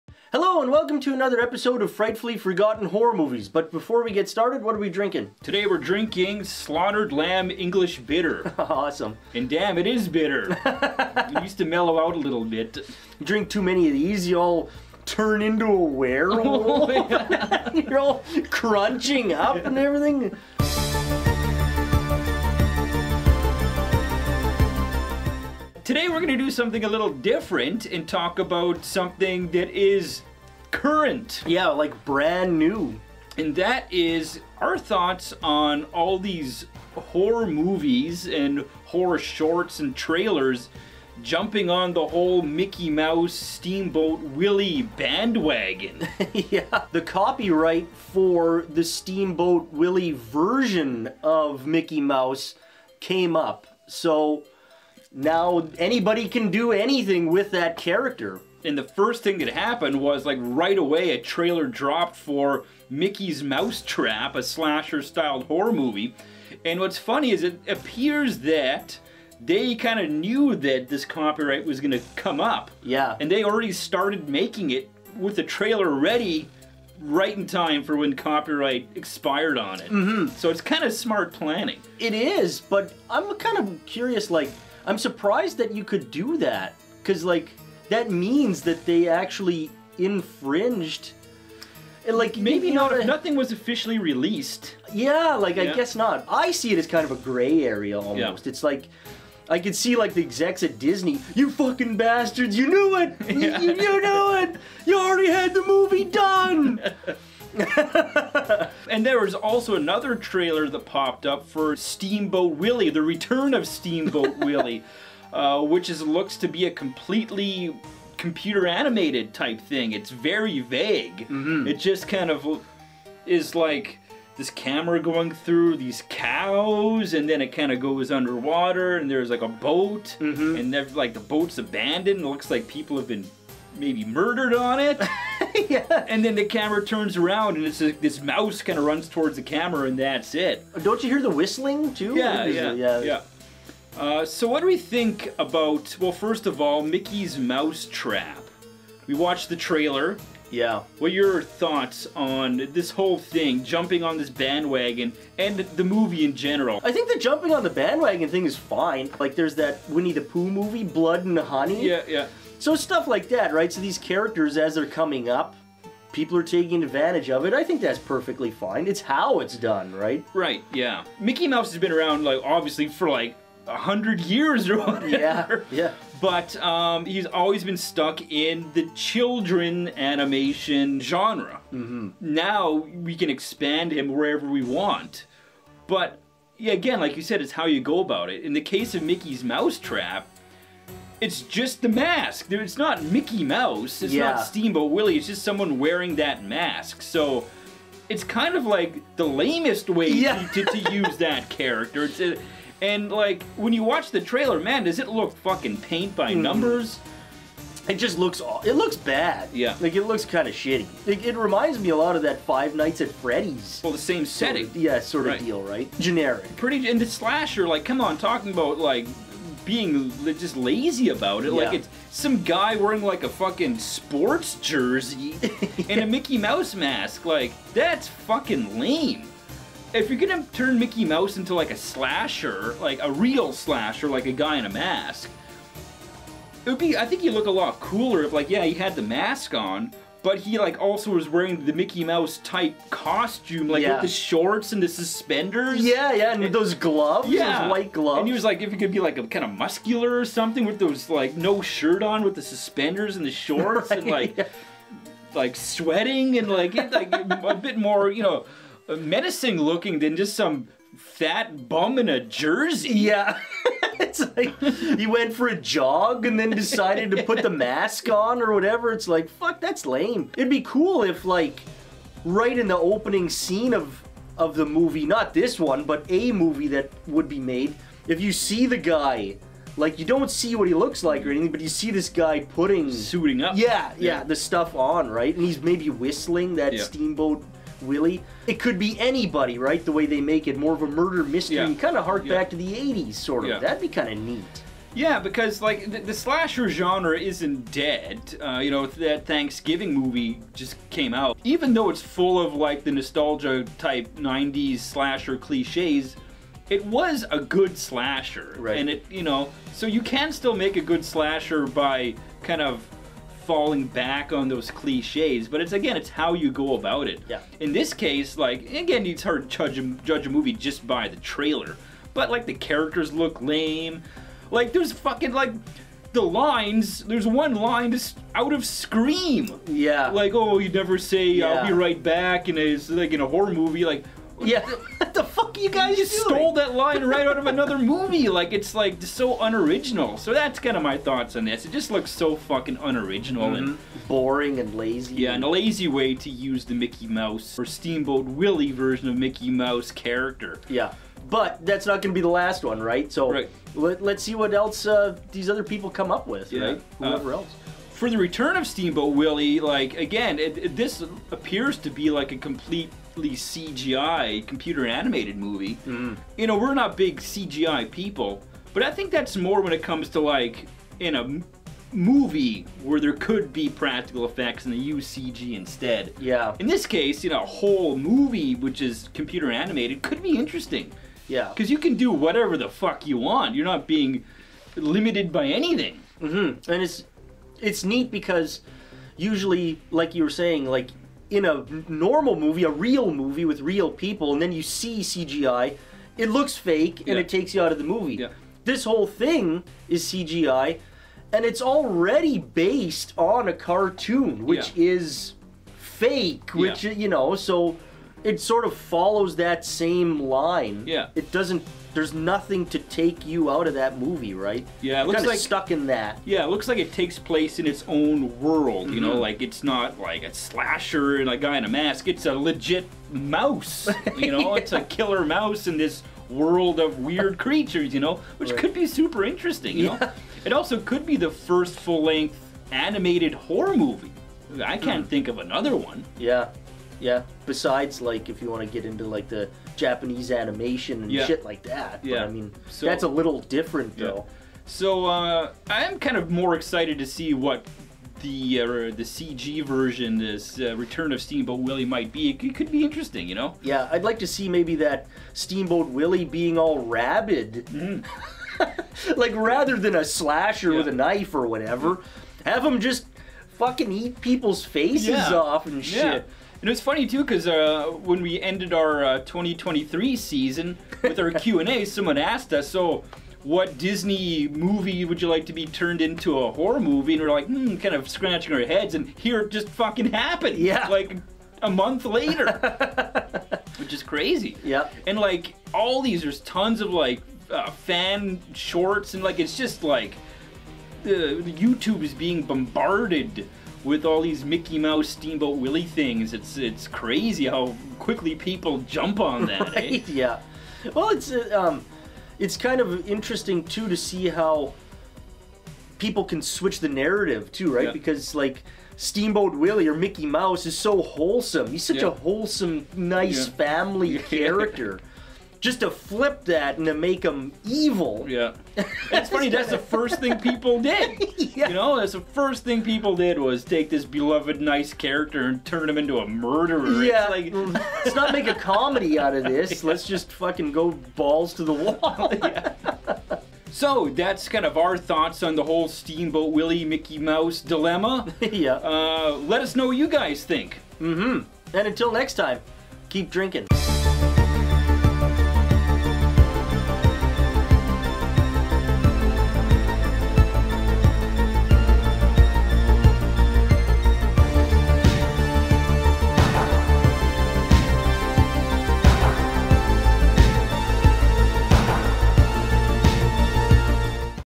Mickey Mouse Is Slashing his way into the Horror Genre!
Steamboat Willie / Mikey Mouse has entered the public domain, and the horror community has jumped on the bandwagon without hesitation. Our thoughts on the trailers and teasers flooding YouTube.
and we will bump up your movie request to the top the the list!